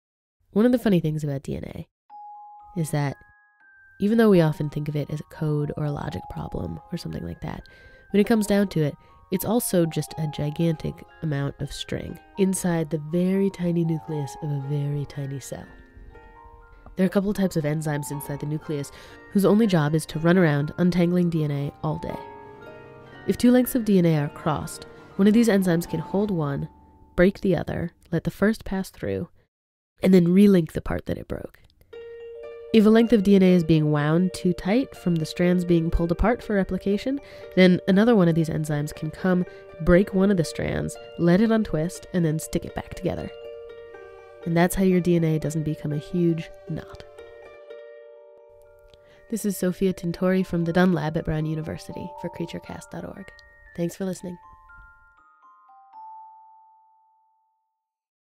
CreatureCast - Tangled String
Video narrated, drawn, and edited by Sophia Tintori, with an original score by Amil Byleckie. Visit CreatureCast.org for more animal stories.